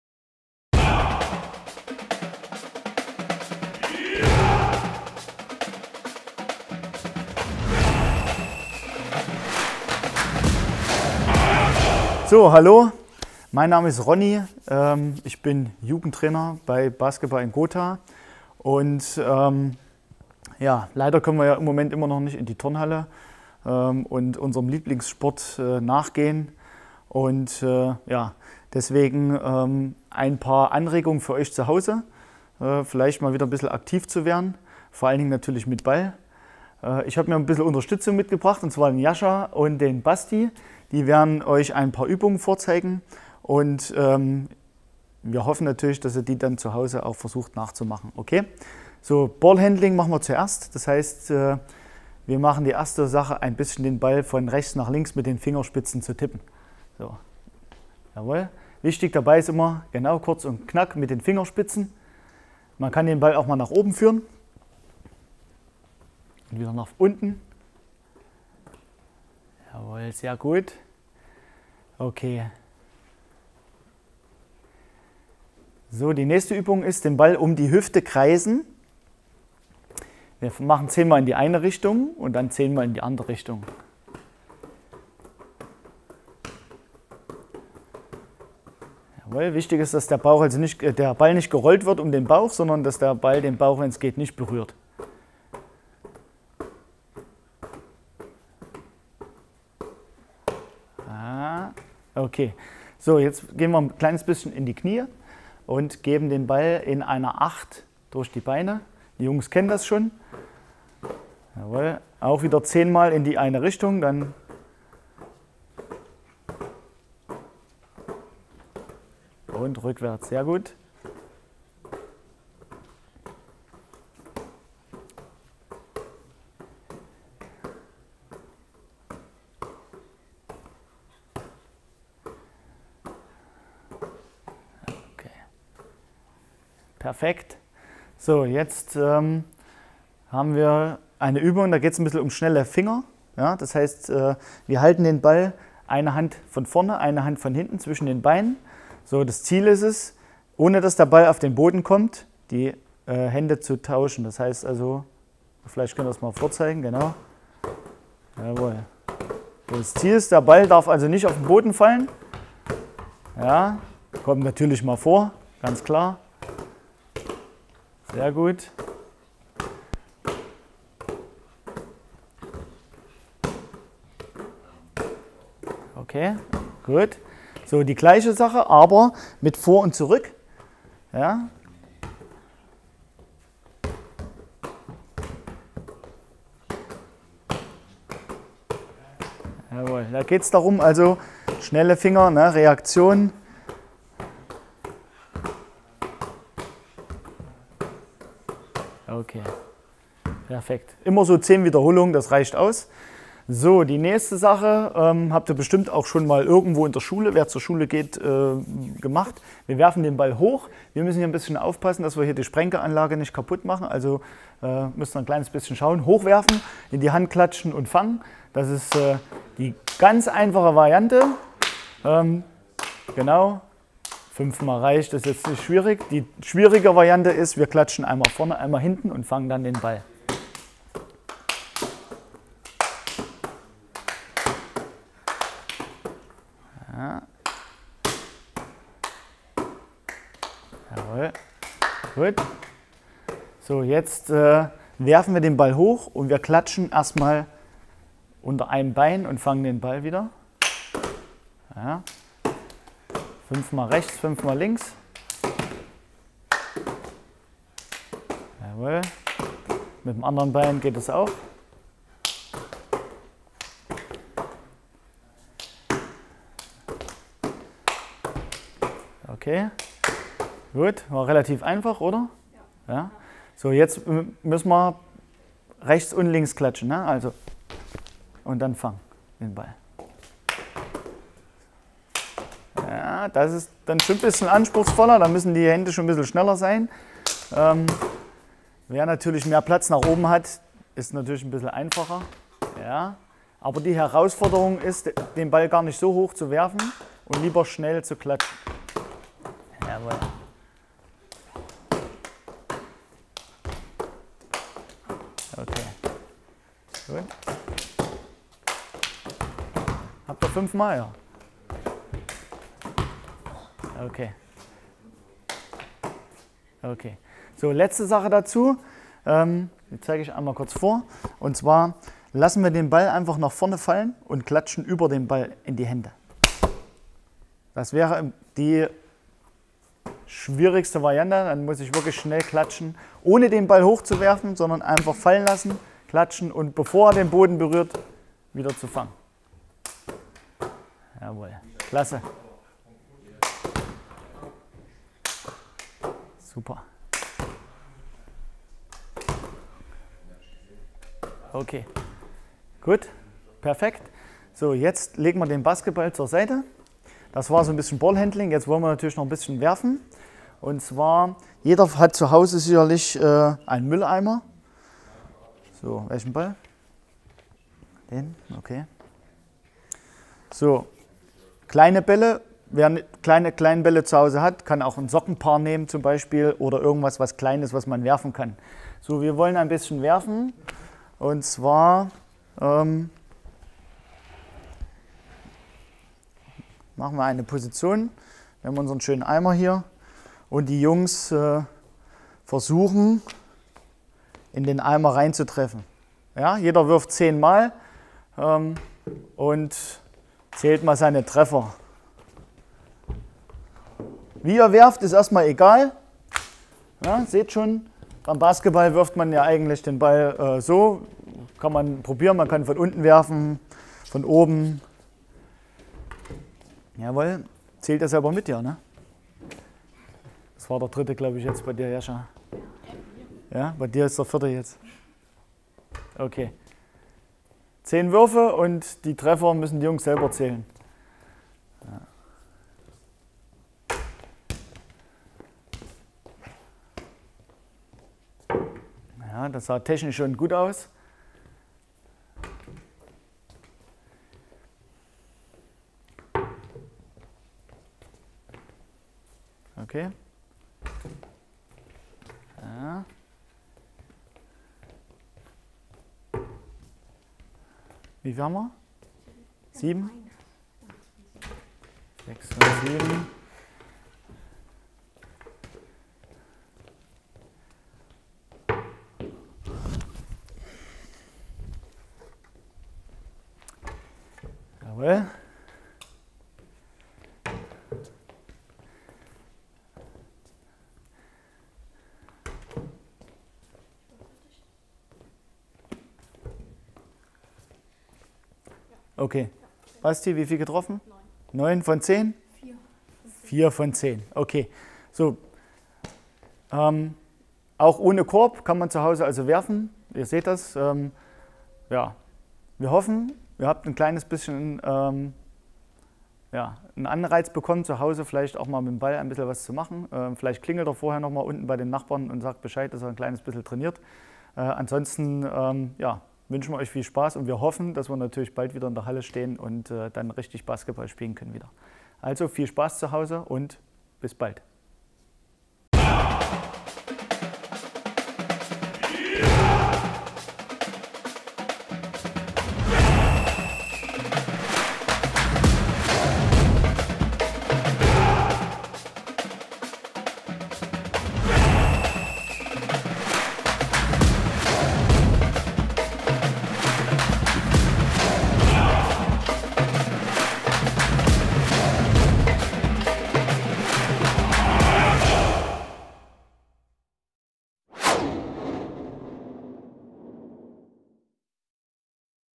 Gotha bewegt – Sportliche Aktion Gothaer Vereine – Folge 2
Sport ist bekanntlich die beste Medizin. Deshalb haben sich vier Gothaer Vereine zusammengeschlossen, um Kindern und Jugendlichen ein sportartübergreifendes, abwechslungsreiches und effektives Trainingsprogramm zu ermöglichen. Dafür wurden kürzlich Videos produziert, in denen jeweils abwechselnd ein Übungsleiter oder Sportler eines Vereins die Schwerpunkte setzt: Mit spezifischen und allgemeinen Übungen verschiedener Sportarten soll ein buntes Bewegungsangebot für den Nachwuchs aller Gothaer Vereine gemacht werden.

Mit von der Partie sind der SV Westring Gotha, der FSV 1950 Gotha, der Marineclub Gotha und Basketball in Gotha.
"Oscar am Freitag TV" und „My Fitness“ unterstützen die Aktion. In der Folge 2 agieren „Ronny, Jascha und Basti“ vom Verein Basketball in Gotha (BiG). Weiter geht es im Takt immer dienstags und donnerstags. Sehen Sie selbst.